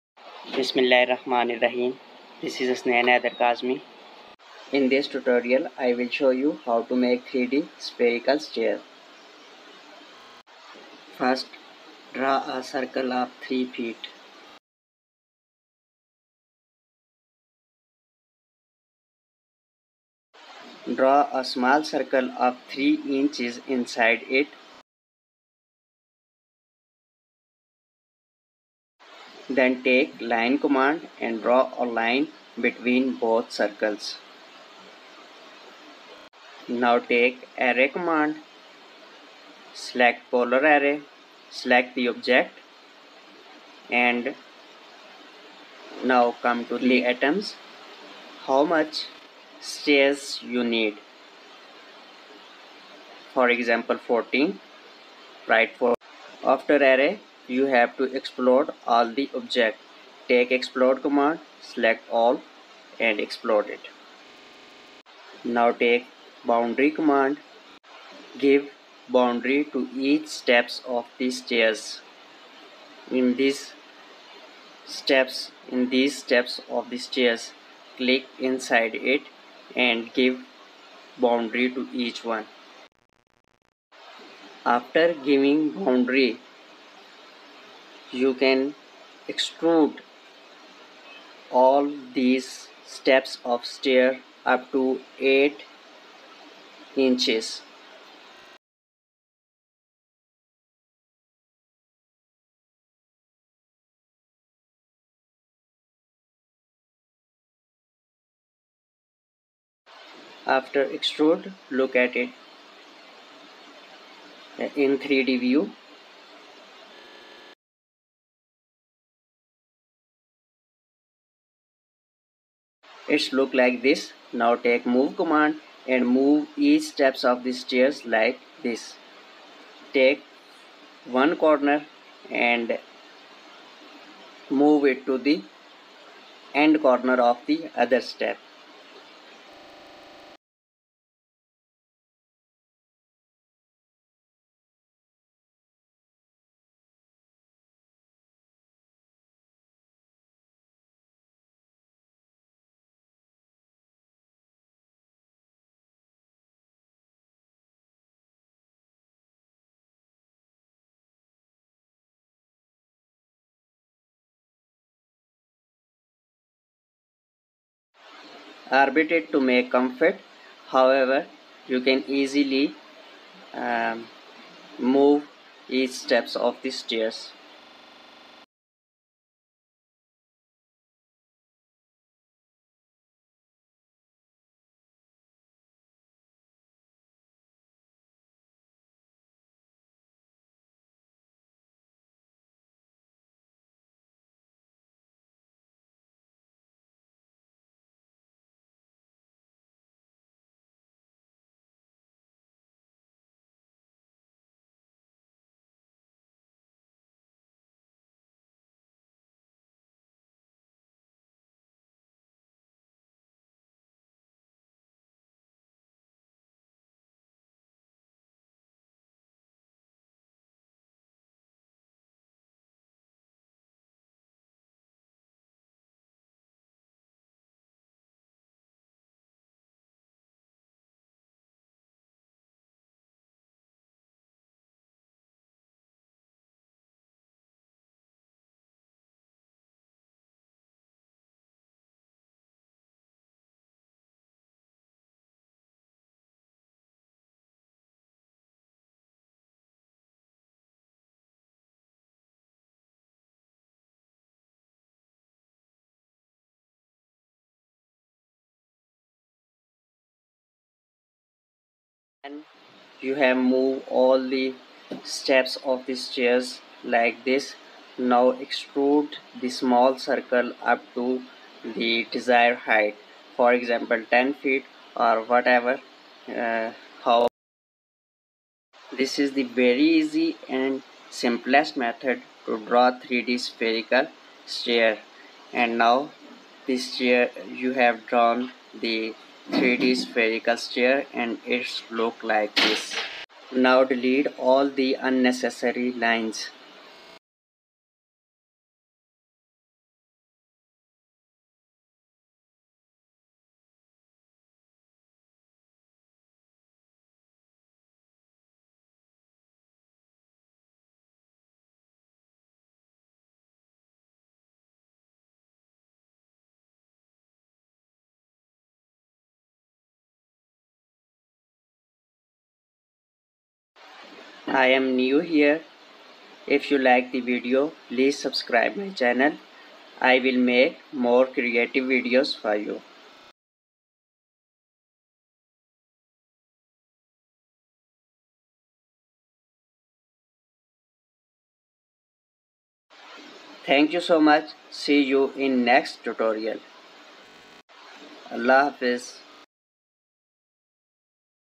3D Spherical Rounded Stairs in autoCAD
In this short video I made a 3D design of spherical rounded stairs in autocad. It is the very simplest and easiest method to create 3D stairs. I hope you will learn from video, will like and enjoy it. I teach the uses of commands in autocad: line, circle, array, explode, boundary, extrude, move and orbit. Hence I teach 3D modelling of stairs in short time.